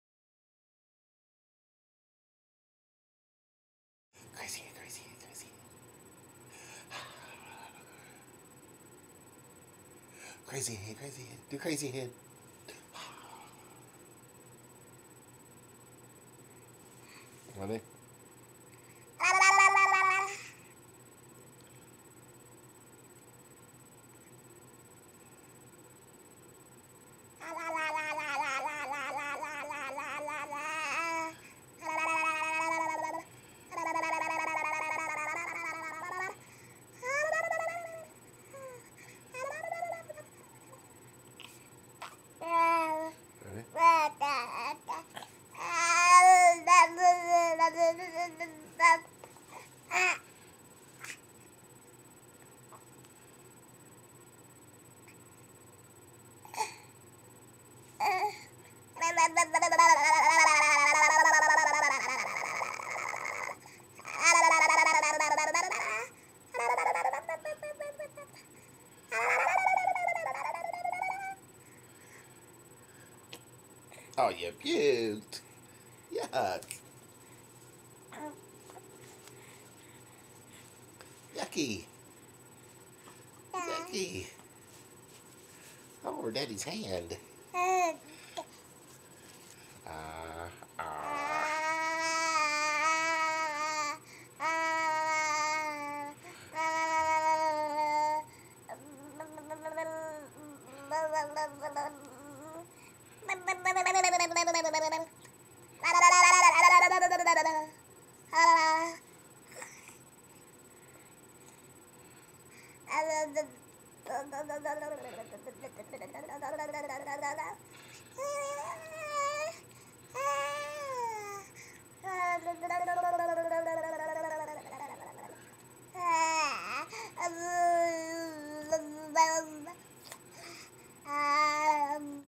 BLUB, BLUB, BLUB
Our baby making funny noises with her mouth